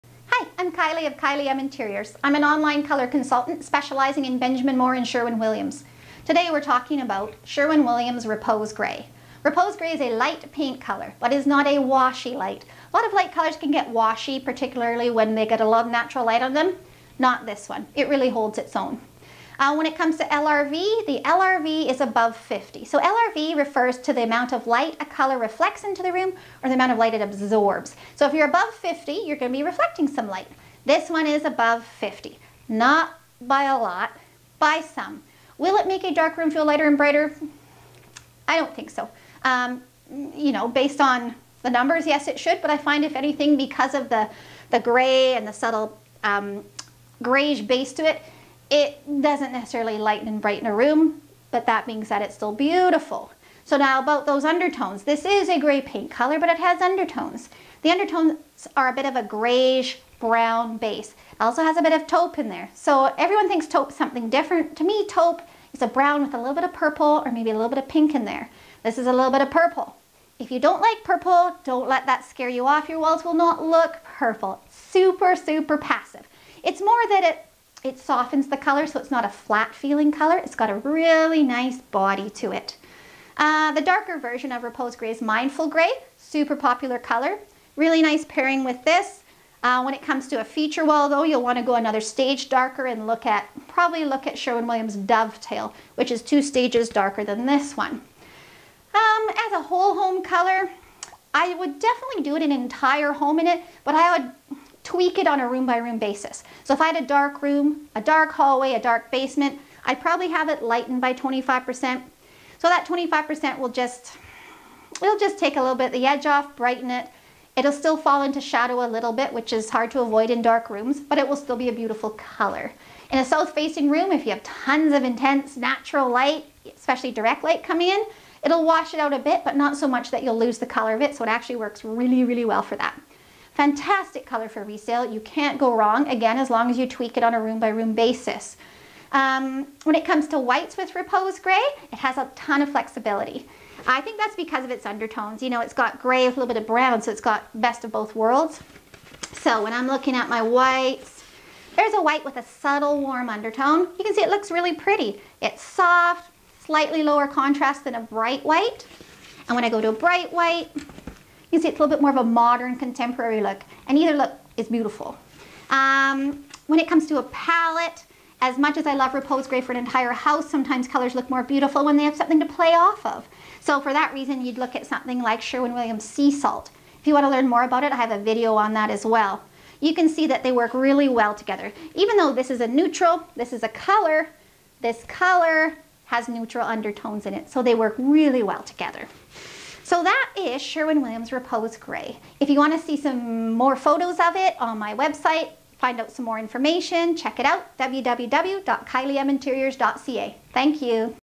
Paint Colour Review: Sherwin Williams Repose Gray SW 7015
Learn ALL ABOUT Sherwin Williams Repose Gray and it's undertones in this video colour review.  Repose Gray isn't a typical cool gray paint colour as it has subtle nuances that you might not see until the painting is done!
In this video review you will learn:
-The undertones of Repose Gray
-How it might work in a north or south facing room
-The LRV and how it can affect a room
Chat soon and thanks for visiting!
~Kylie